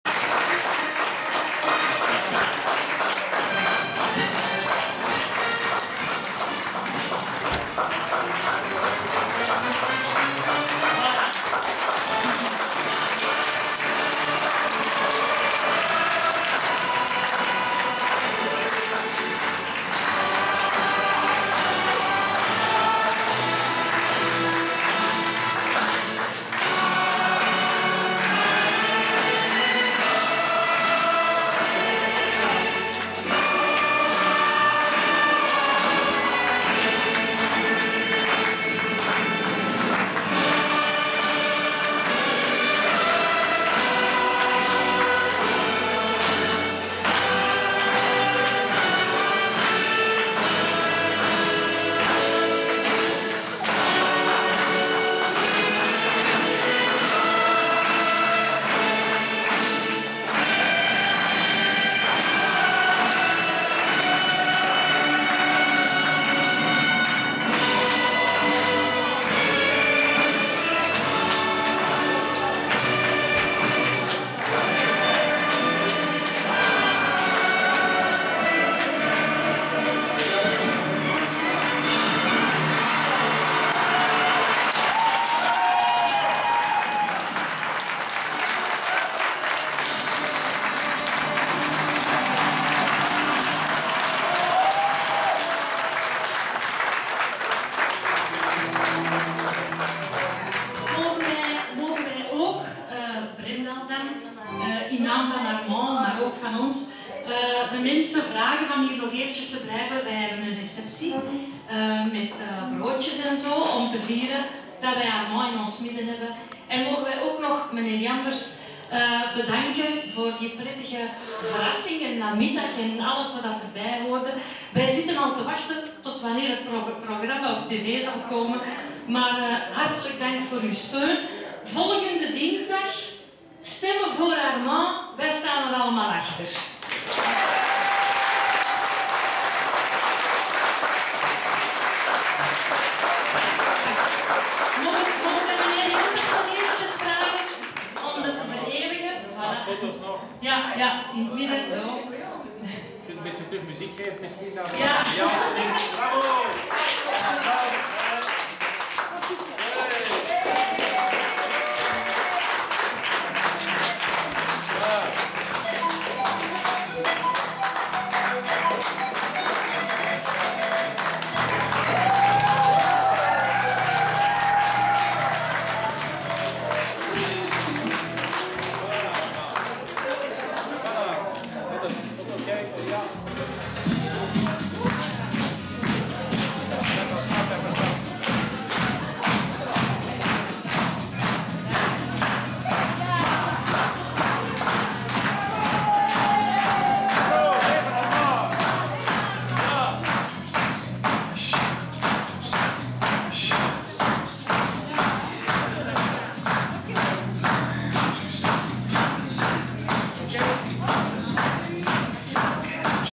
Armandke Delen showt, Paul Jambers filmt in Bremdael RVT Herentals / ThalsFM HD
Armand Delen showt, Paul Jambers filmt in Bremdael Herentals - Anne-Marie Van Dessel